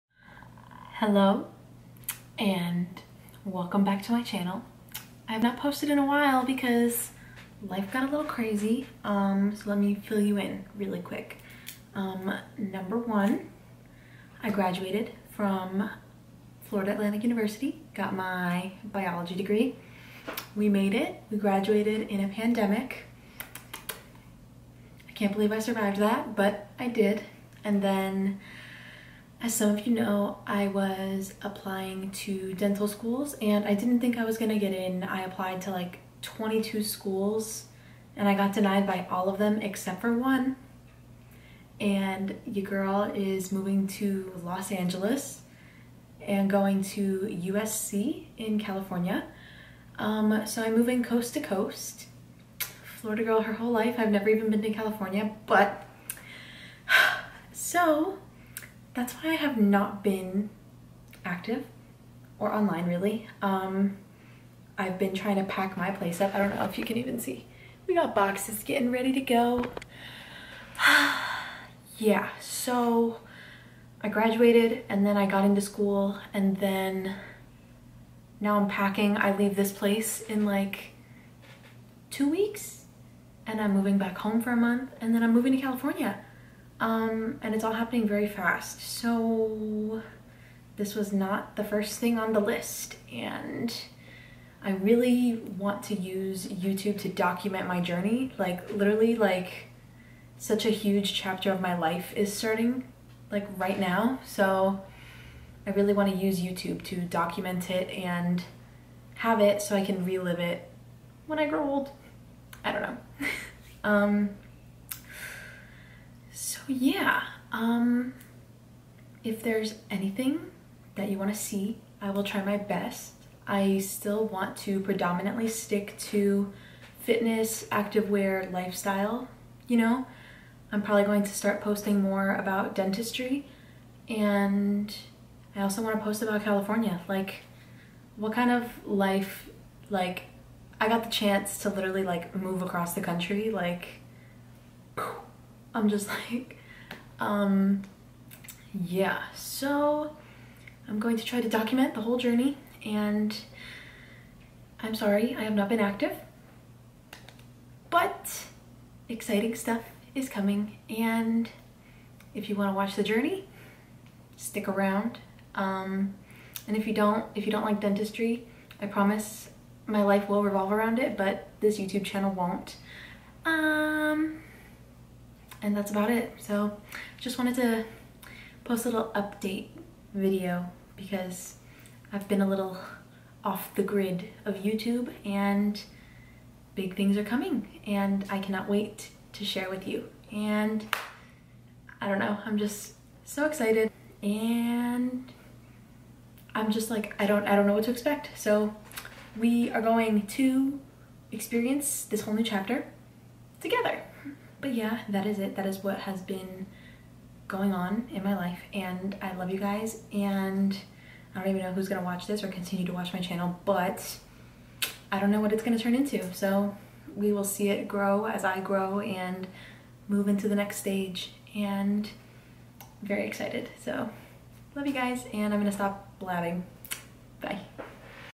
Life Update! Graduation, Moving, Dental School?!
Hello everyone! I just wanted to hop on and give you all a quick update of what has been going on in the past few months! A lot of crazy news, and I can not wait to take you all along on this journey! If there is anything you want to see, please comment it down below! I love you all so much and thank you for your support!